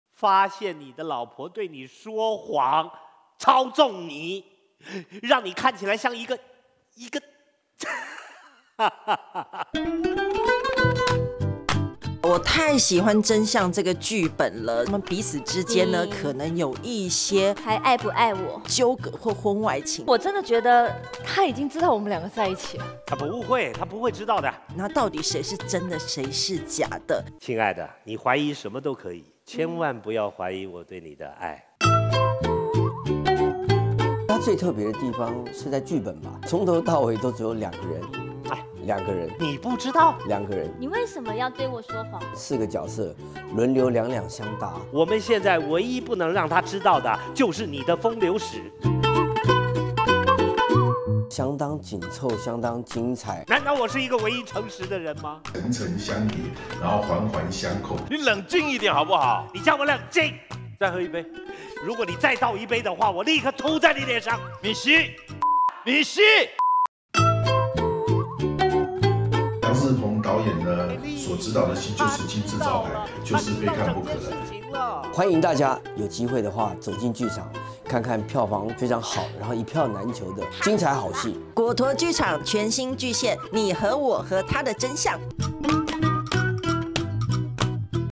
2024《妳和我和他的真相》口碑一致盛讚，精華影片大公開！
2024萬眾期盼，原班人馬，重磅回歸！

⭐屈中恆  ⭐賴雅妍  ⭐蔡燦得  ⭐劉亮佐
     影帝影后級演出火力全開💥

「誠實是種美德，謊言又那麼罪不可赦嗎？」
從第一場床戲到最後一場吻戲，笑聲驚呼此起彼落，毫無冷場，
真相像洋蔥一樣一層層被剥開，劇本精彩玄妙，令人拍案叫絕！

★《最後14堂星期二的課》國際導演 楊世彭 打造深度喜劇全新製作
★ 英國、義大利、西班牙、德國、澳洲、香港多國改編巡演，口碑熱議，台灣首度登上劇場！
★ 最荒誕絕妙的當代婚姻寓言；最令人猜不透的懸疑婚姻喜劇。
★ 在愛裡，我們選擇善良。

劇情緊湊，言詞激烈交鋒💥，妙語如珠連環炮🧨，首演後口碑沸騰🔥，眾人齊聲推薦：

曾國城👉「四位精彩演員用多年的情誼去推砌隨時有可能崩解的真相，而人性真實和虛偽又代表什麼？非常過癮的一齣戲。」
卜學亮👉「整齣只有七場戲，一場接一場，層層相疊又環環相扣，每場戲都在看影帝影后飆戲，越看越精彩！」
唐從聖👉「相當緊湊、精彩，充滿愛恨情仇的喜劇，兩個半小時很快就過去，意猶未盡。面對另一半的愛、信任與背叛都有很有趣的辯證。真的要給四位演員鼓鼓掌，太厲害了！」
楊千霈👉「到底什麼時候該講真話，什麼時候要有善意的謊言？在婚姻裡的你會心有戚戚焉，必看的經典喜劇。」
趙小僑👉「太喜歡這個劇本了！人生不免有些悲傷但又非常荒謬，偏偏在外人看來又十分好笑，真的太玄了，歡迎進劇場體會！」

別錯過 果陀劇場 精采好戲《妳和我和他的真相》💯